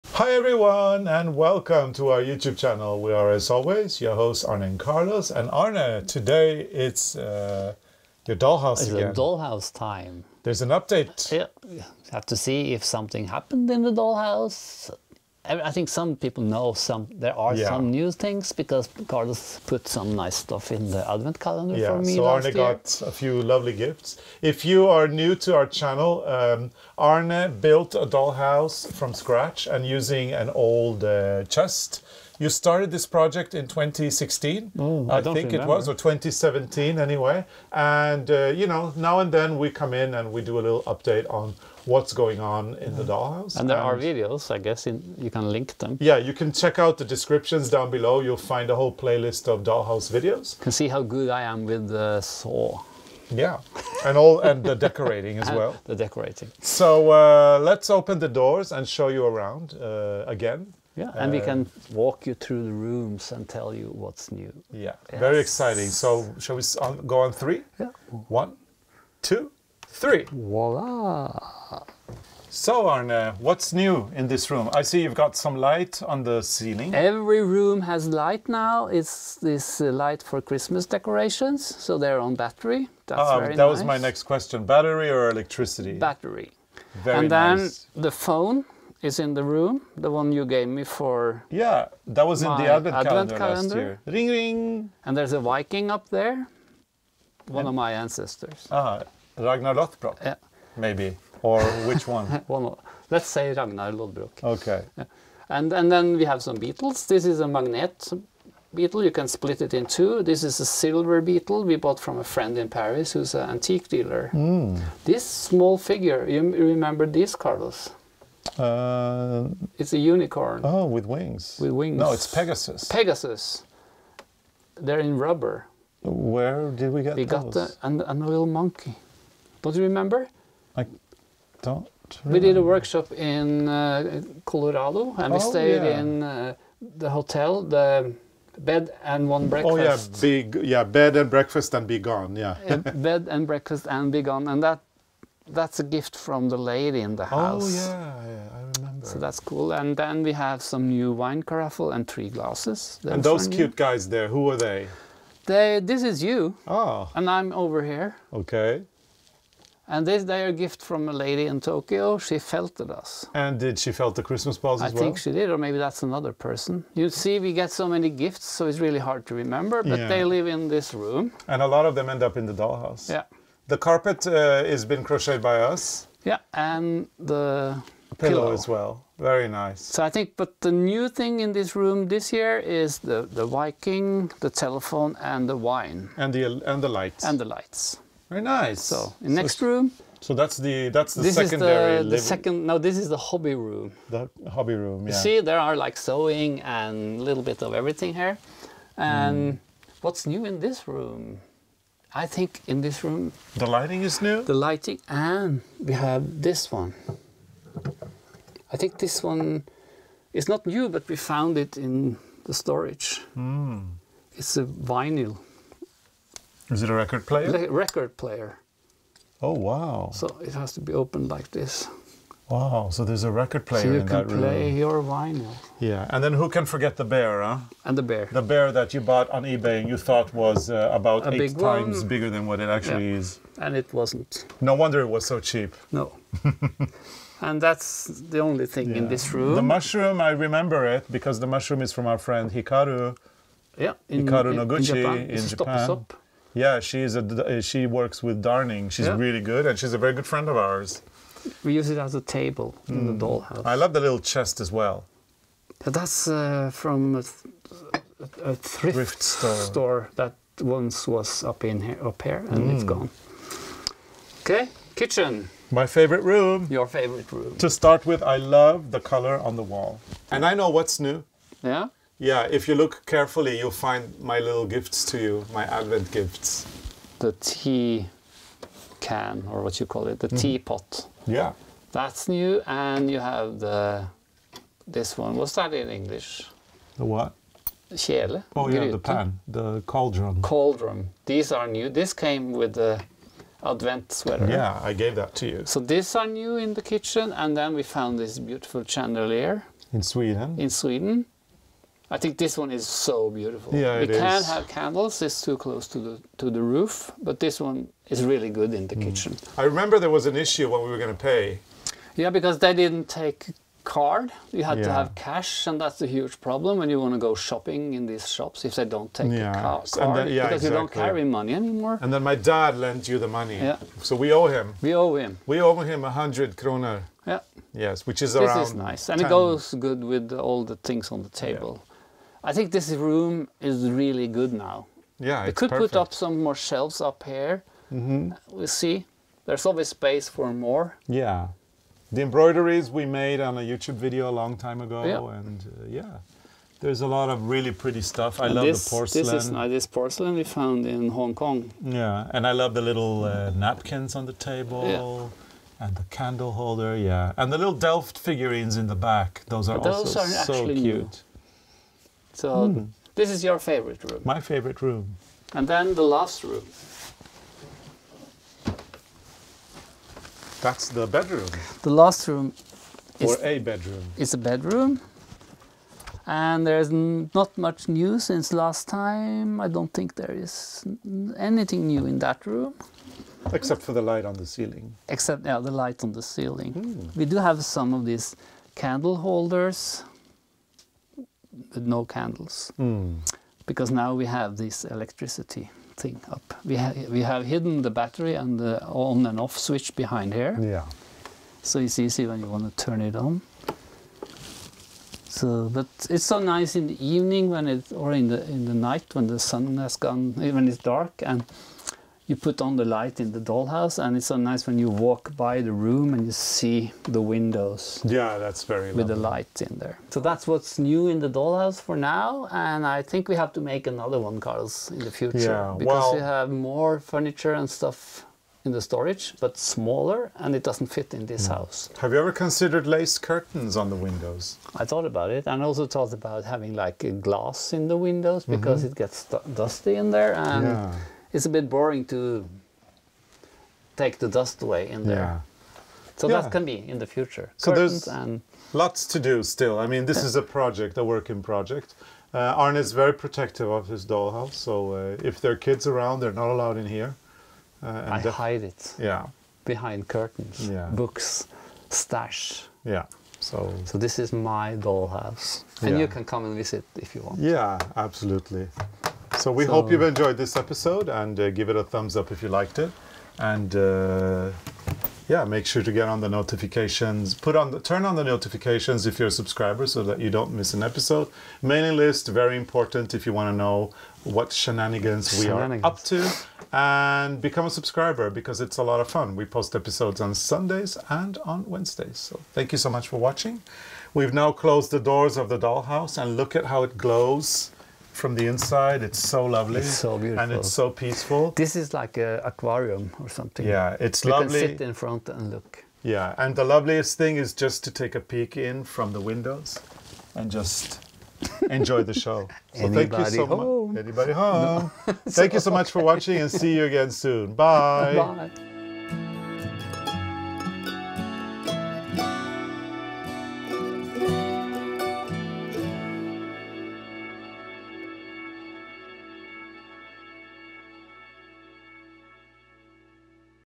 Dollhouse - by ARNE & CARLOS
We are back from our summer vacation, and this week, were going on our latest house tour to see the newest updates from our dollhouse.

We publish a weekly, new tutorial every Sunday on both our YouTube channel and our Facebook page. We hope you enjoy it!
ARNE & CARLOS

  arnecarlos